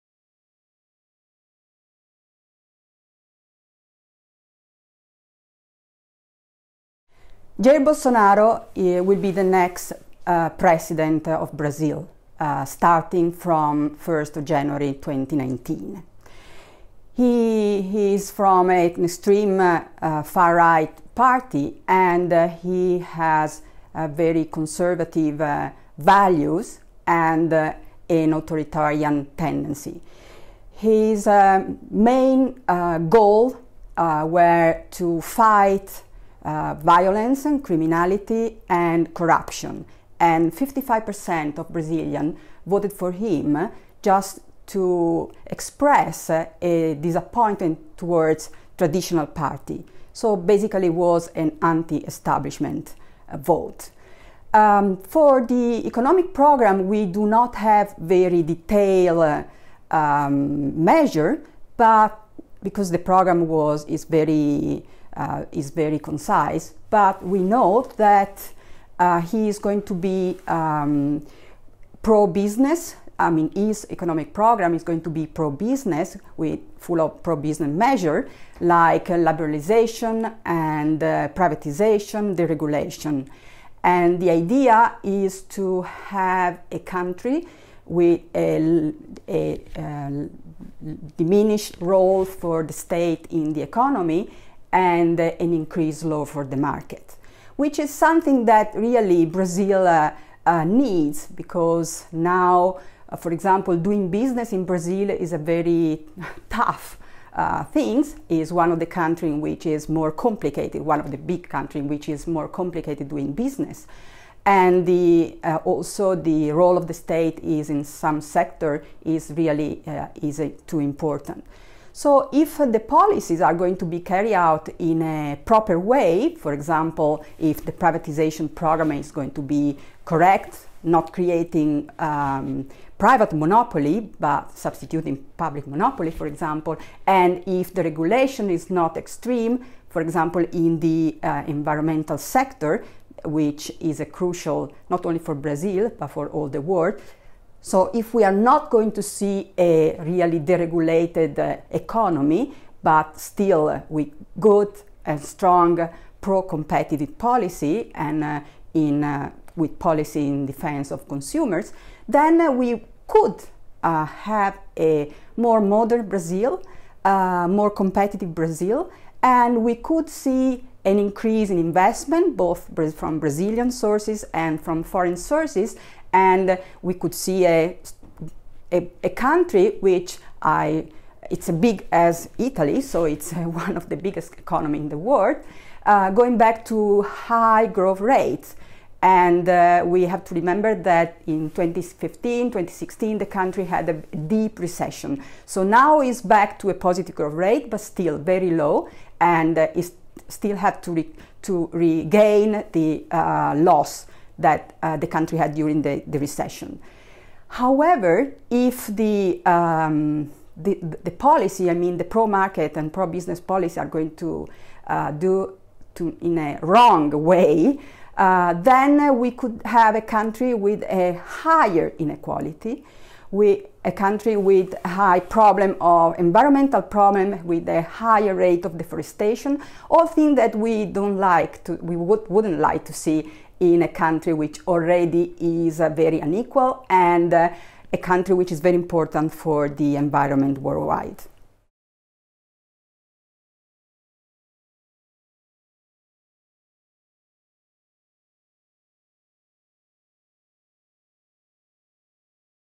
Video/Outlook for Brazil’s Economy with Bolsonaro
Liberalizations, privatizations, deregulation - Jair Bolsonaro has promised pro business policies. Done in the right way – not creating private monopolies, not deregulating the environmental sector and with pro competition policies for example - it could render Brazil’s economy more modern and competitive, explains in this video Antonella Mori of the Department of Social & Political Sciences. Done in the wrong way, the policies could increase inequalities in the country or further damage the environment.